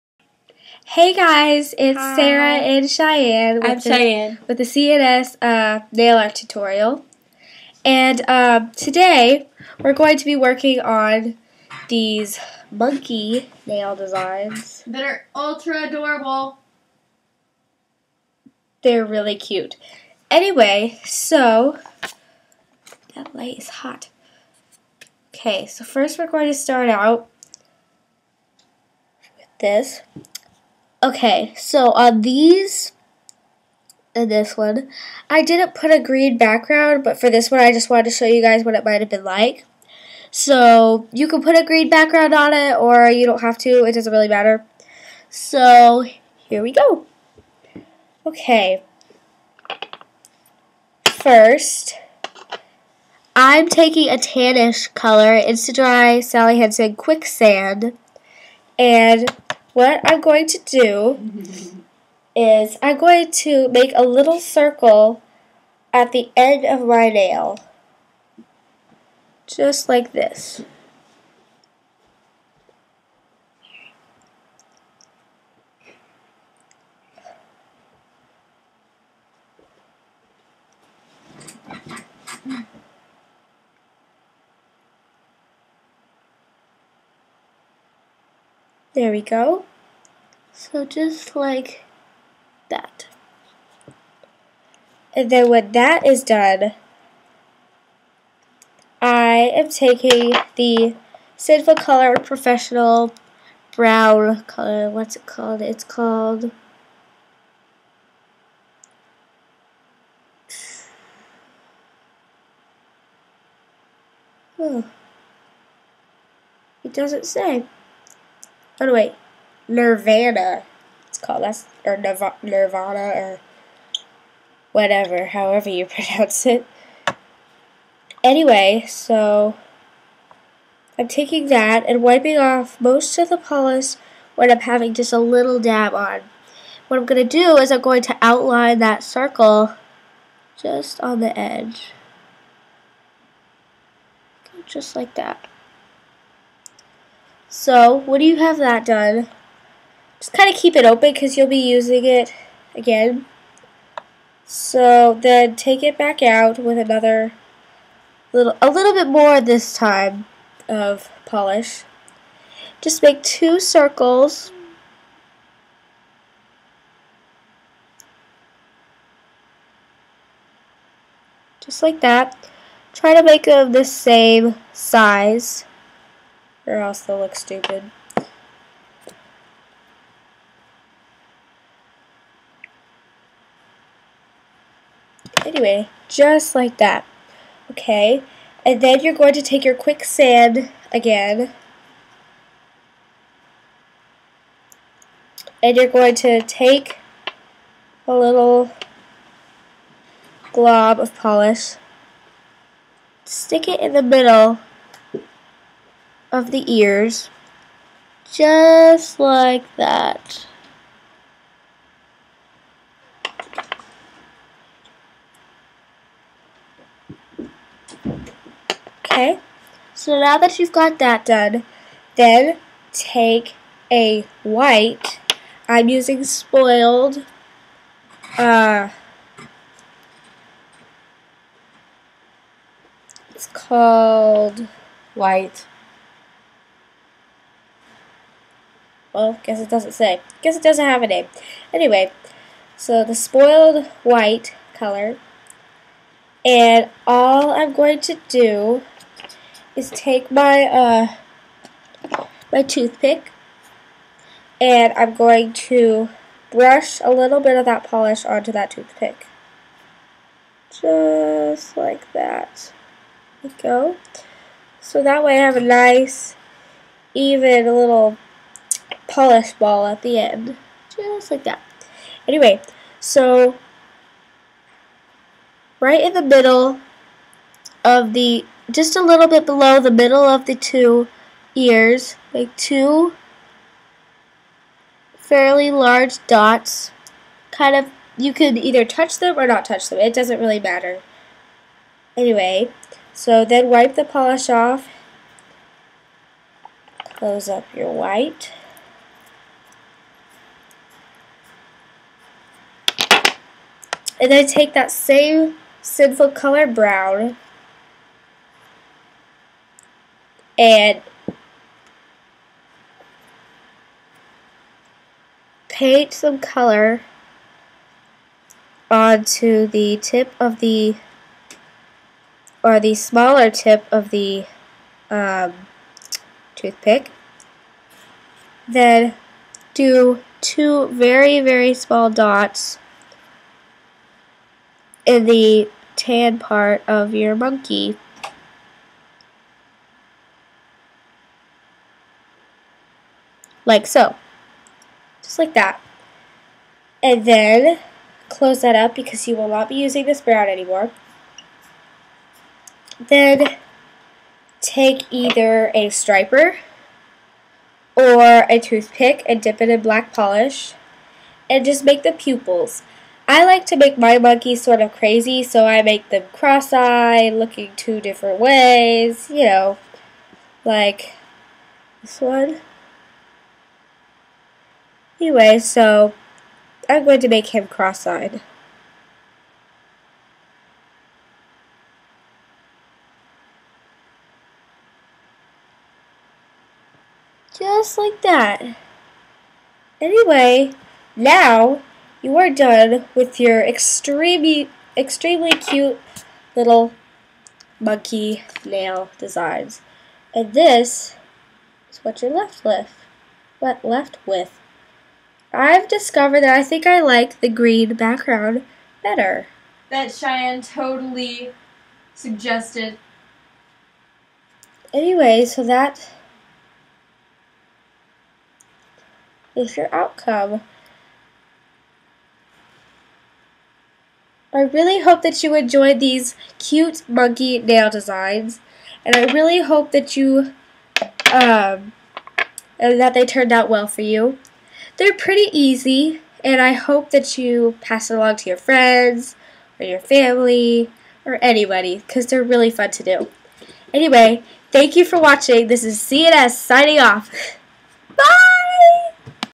Monkey Nails
Hello guys! We hope you are all doing well! This cute monkey nail design is so appealing and it cannot go wrong. They are pretty easy and the materials that you use are very common. As we had said in the video you can add a green backround to the monkeys or just leave your nail plain. We thought the green background was cute so we decided to show you both.